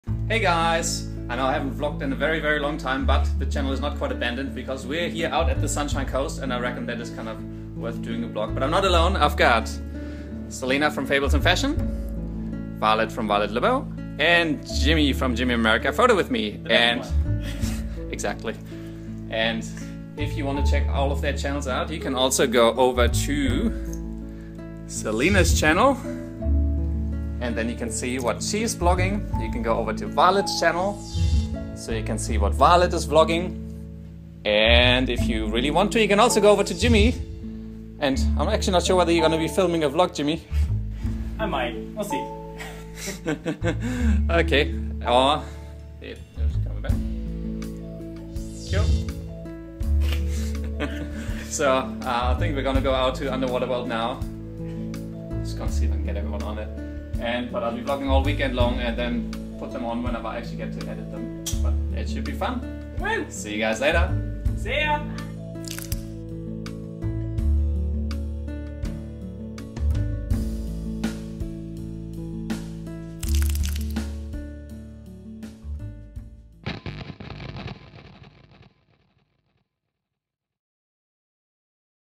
Sunshine Coast Vlog #1 - Intro
I went to the Sunshine Coast with Celina, Violet and Jimmy and decided to capture our adventures in a daily vlog :)

So enjoy summer, sunshine and lots of things to do and see!